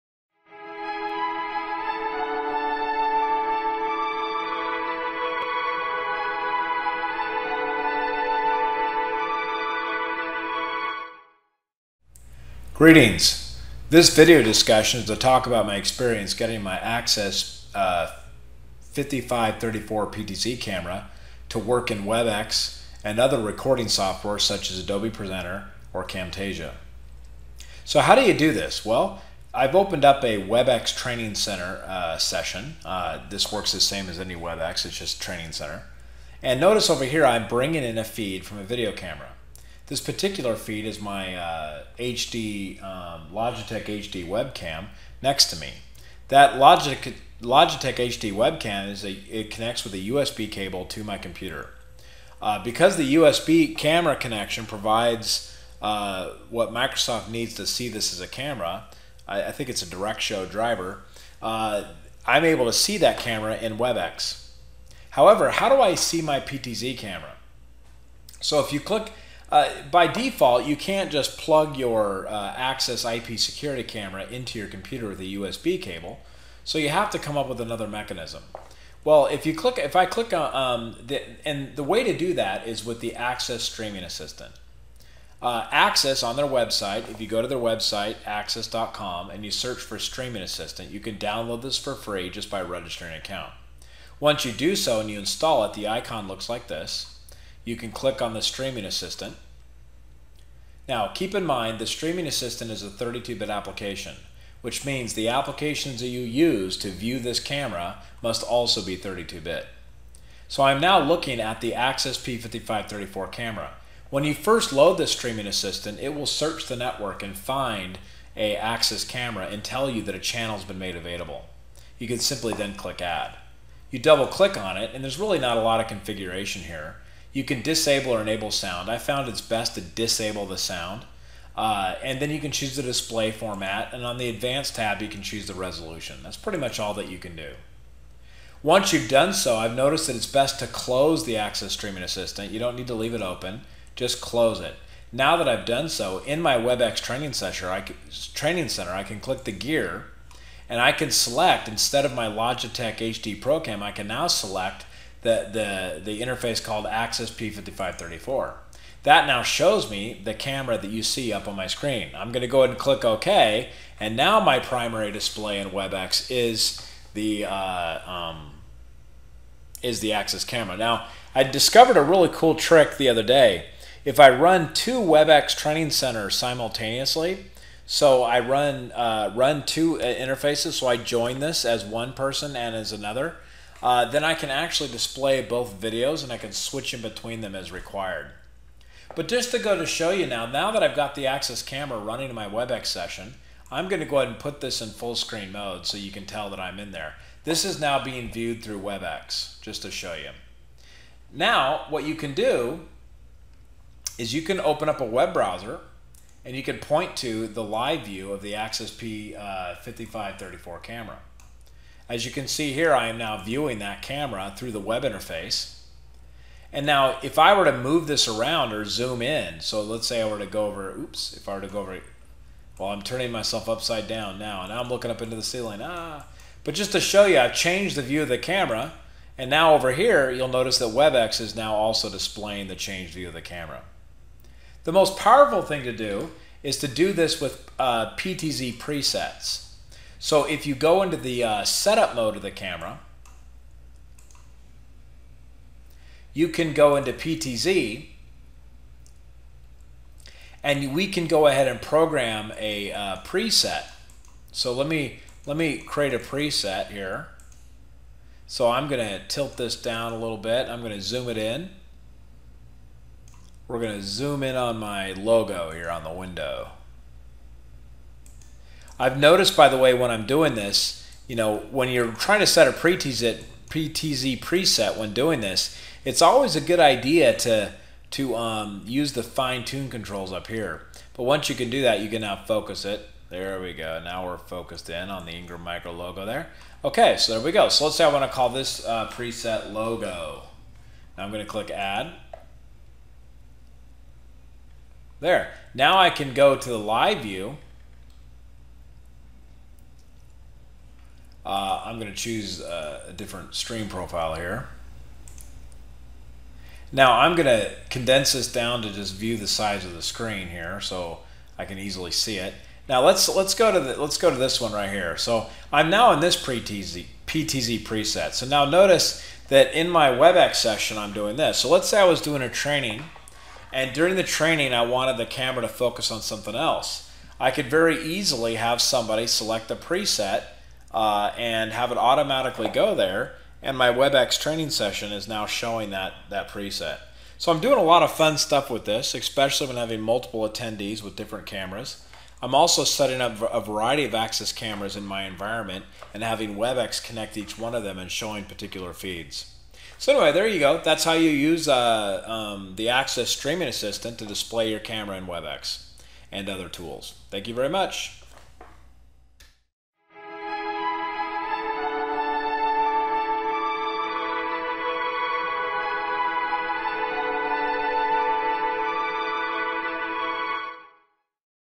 PSU: How to get an Axis 5534 PTZ Camera to work in WebEx
Using Axis Streaming Assistant